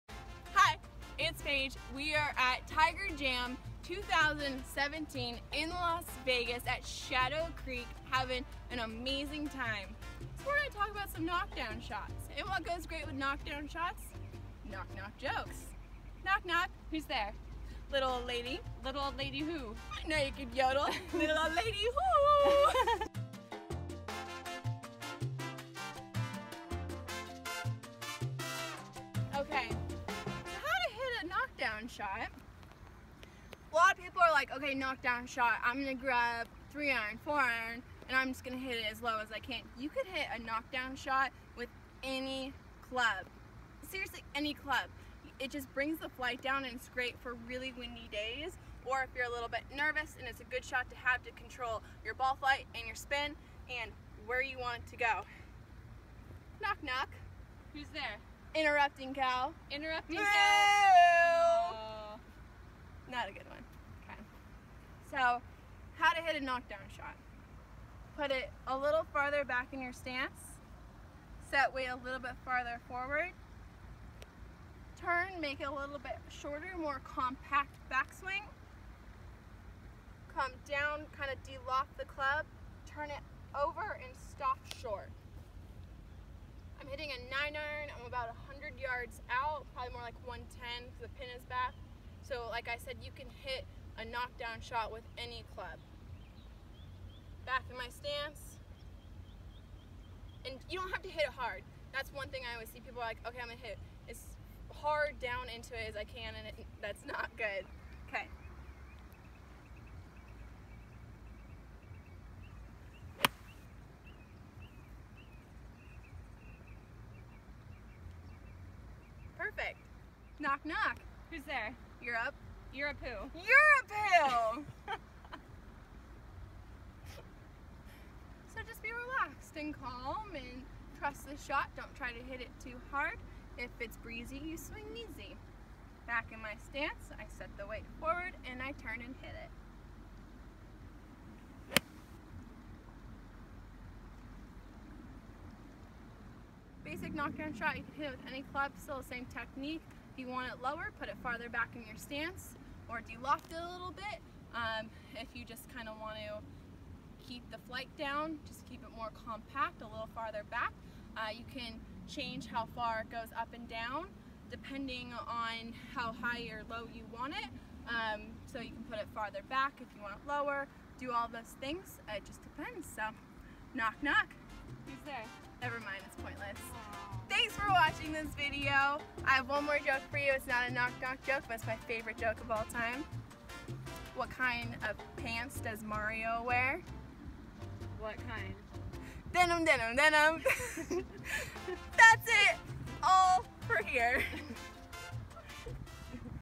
How to Hit a Knockdown Shot // Golf Tips with Paige Spiranac // Shadow Creek
If want to learn how to hit knockdown shots with a side of questionable knock-knock jokes, this one's for you! Which club to pick, how to swing, when to hit it, I talk about everything you need to hit this shot. (I was maybe having a little too much fun while filming this during Tiger Jam at Shadow Creek Golf Club in Vegas... PS, subscribe to my channel!